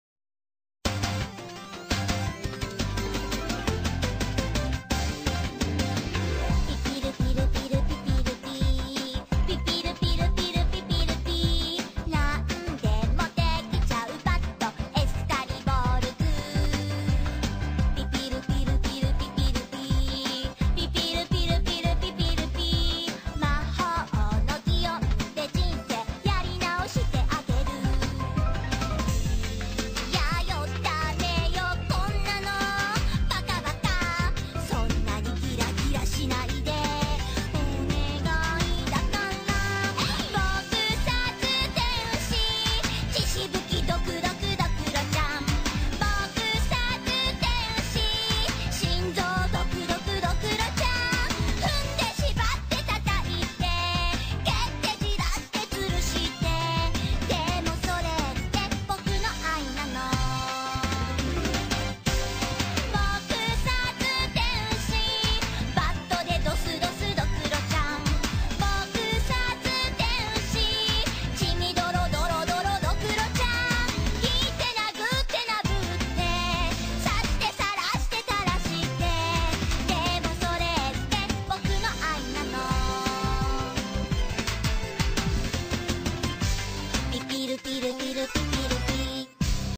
Dokuromon
This is Dokuromon, Scorpionmon & Hobiemon's cute, but deady, daughter. 8D

I based her mainly on the main character of Club-to-Death Angel Dokuro, Dokuro. I've only seen one episode with a friend and itis funny  but bloody too with some other things. XD Pretty much Dokuromon is just like Dokuro they even have the same bat but I forgot to draw it. ^^U

Enjoy and Merry Christmas! 8D

Her Forms:

Fresh: Kuumon
In-Training: Dokumon
Rookie: Dokuromon
Champion: Zankuromon
Ultimate: AngelZankuromon
Mega: DeathAngelZankuromon
Armor: Excalimon - Destiny
Kurygamon - Miracles